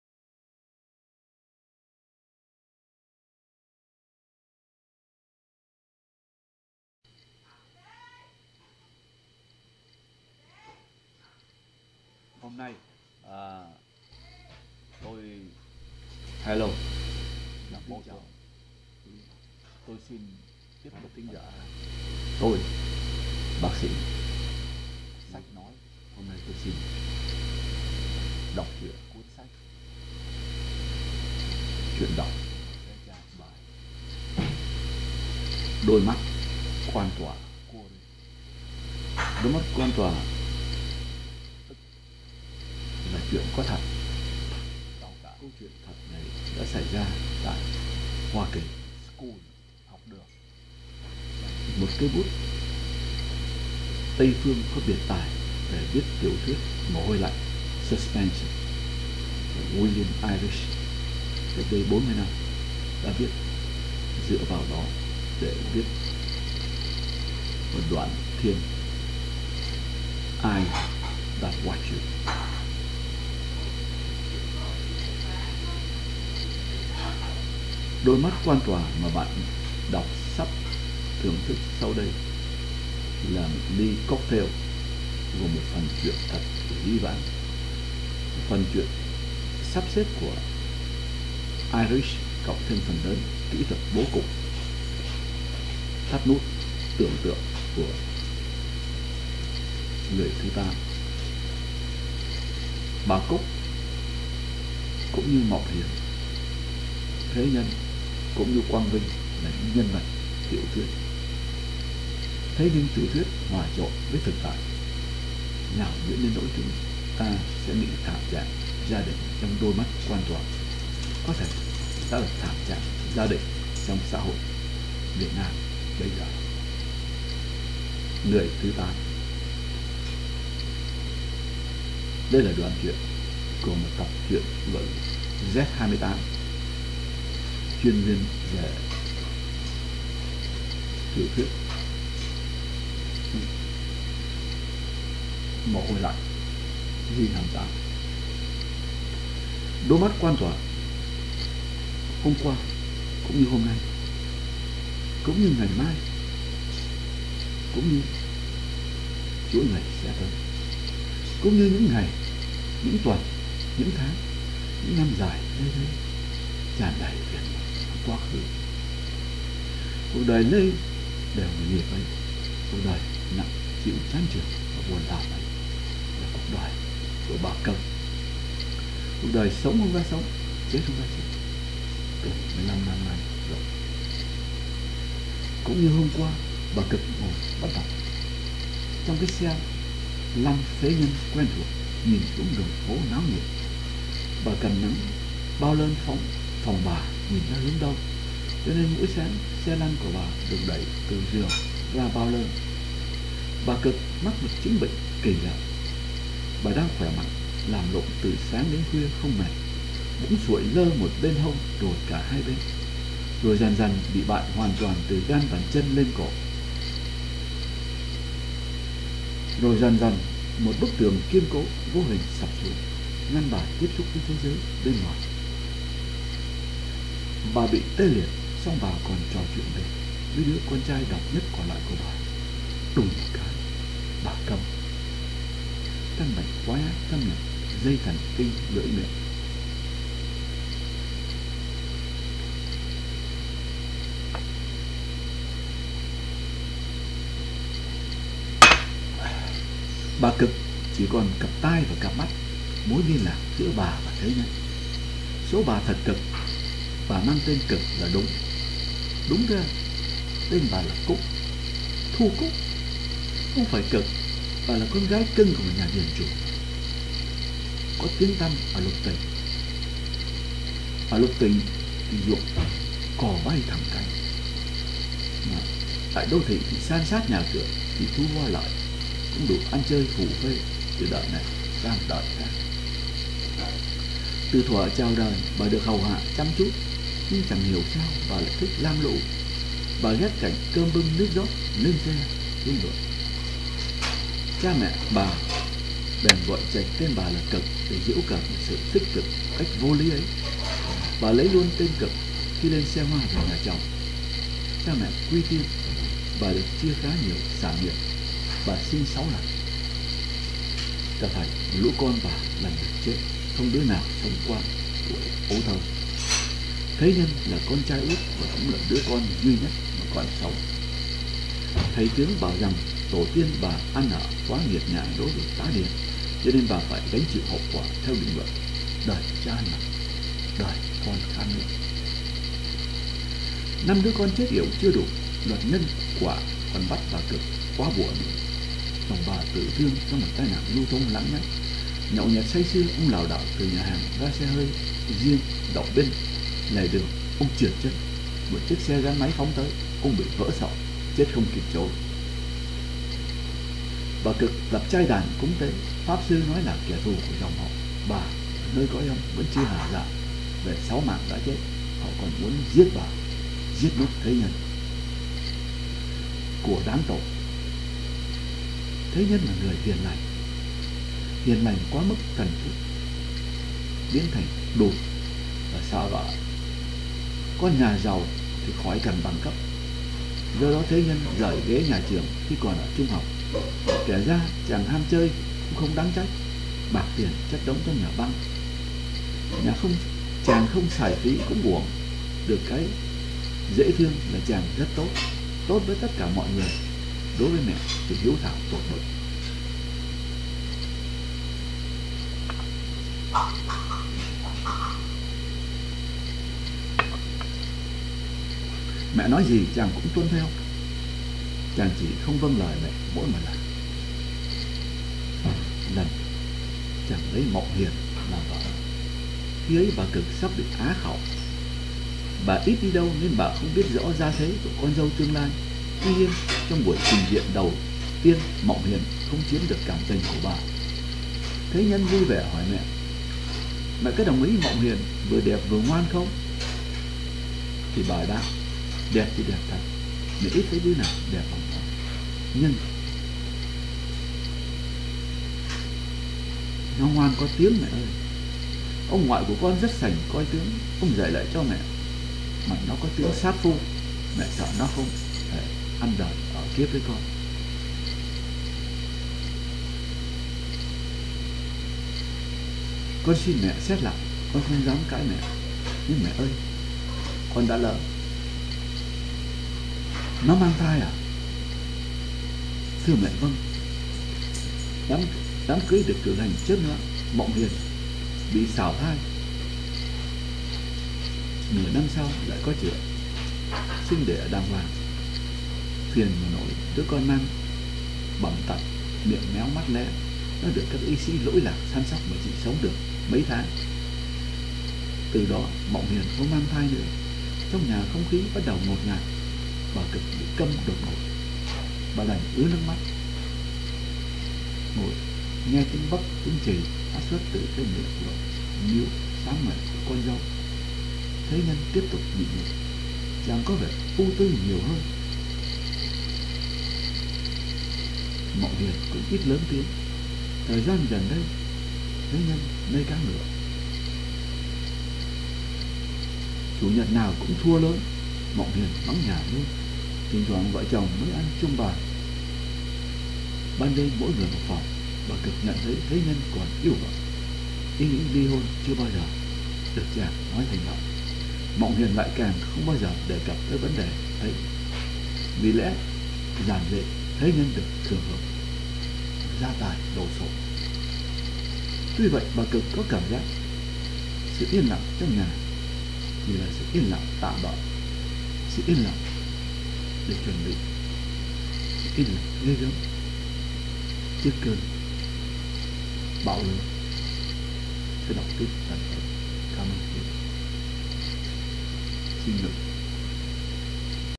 Doi mat quan toa ( The eyes that judge you ).
This is the audiovisual story of a suspense story , The eyes that judge you, read by me from a suspense, crime thriller story novel ./.